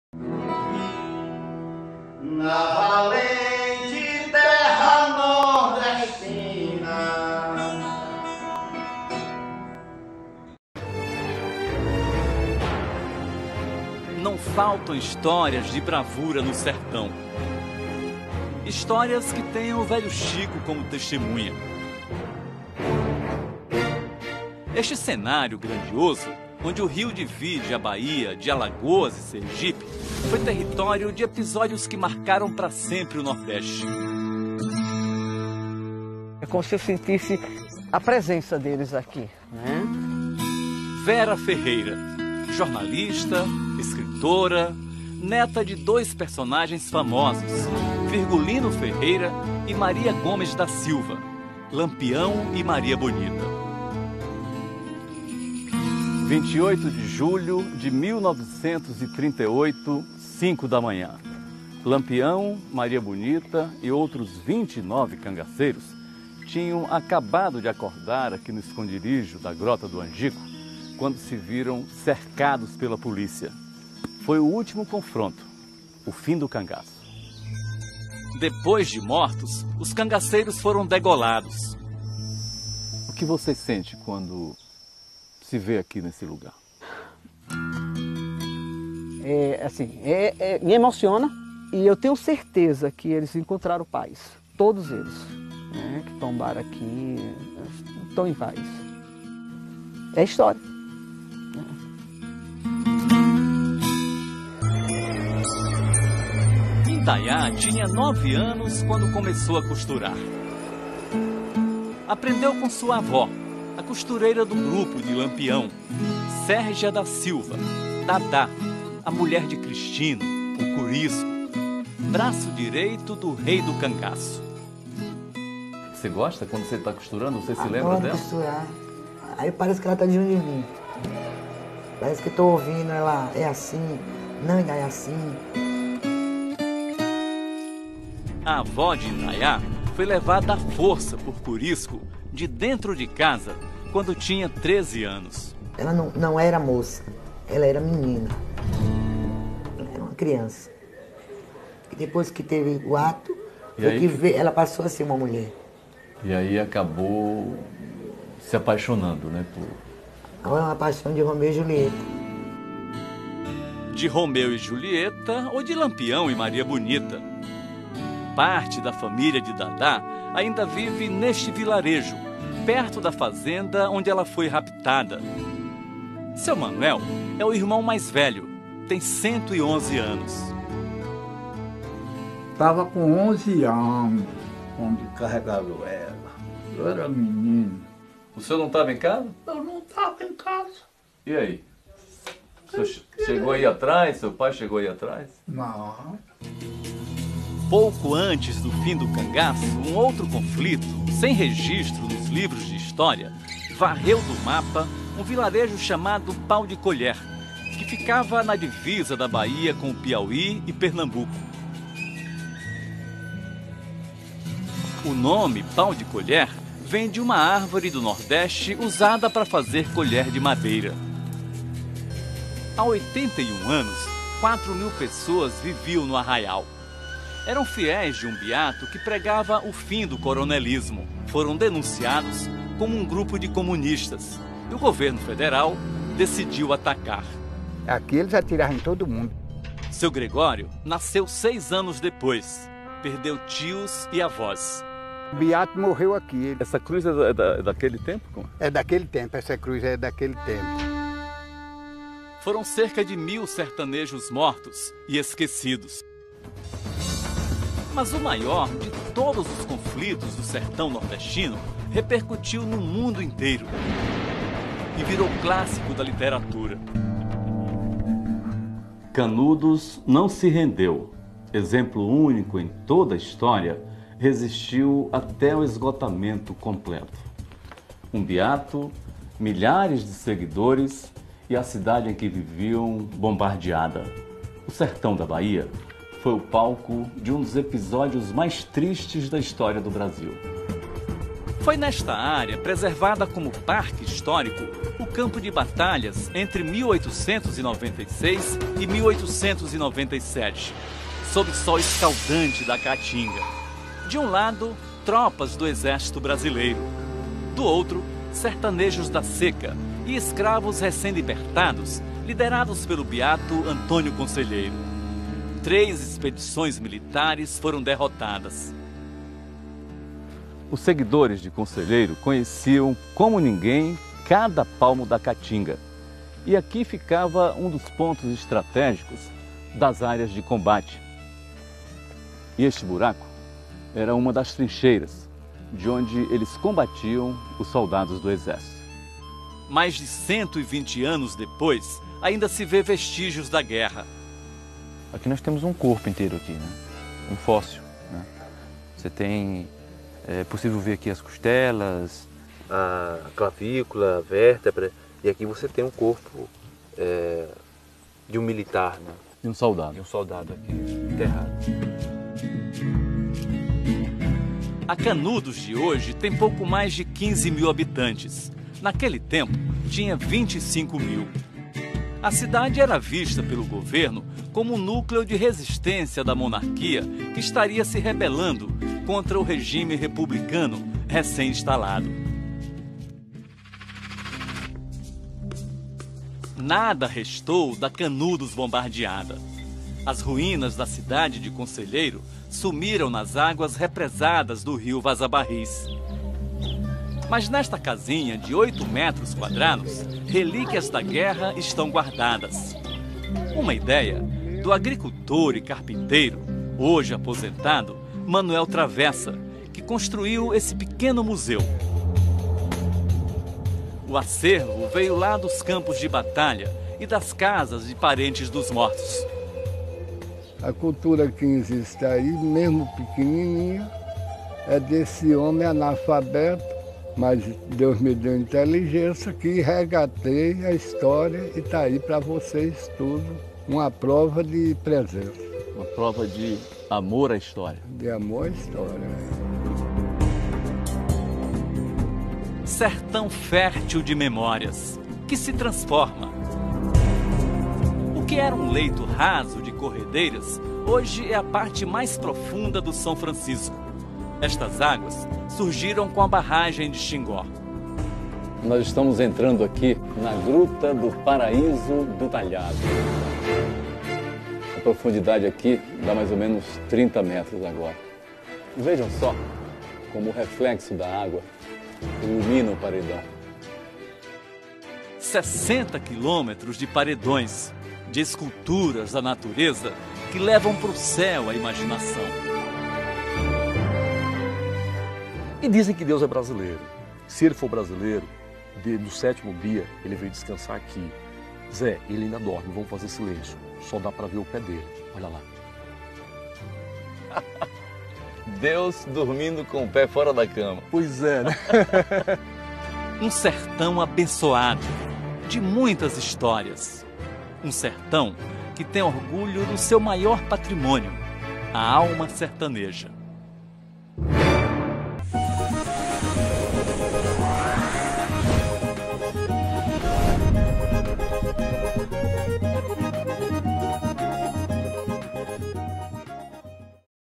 Documentário Sobre Lampião
Reportagem Fantástica! Mostra como está atualmente o cenários por onde o bando vivia.